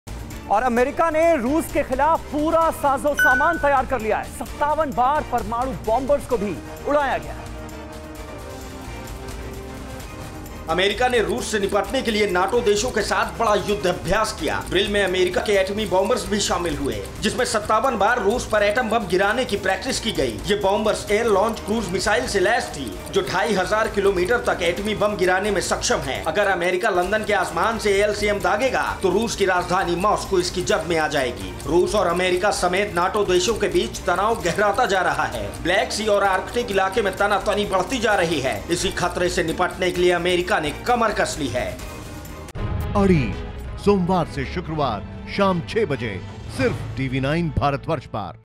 रूस के खिलाफ US की वॉर प्लानिंग.. लंदन से रूस को टारगेट
57 बार उड़ाए परमाणु बम.. उड़े रूस के होश.. रूस के खिलाफ US की वॉर प्लानिंग.. लंदन से रूस को टारगेट..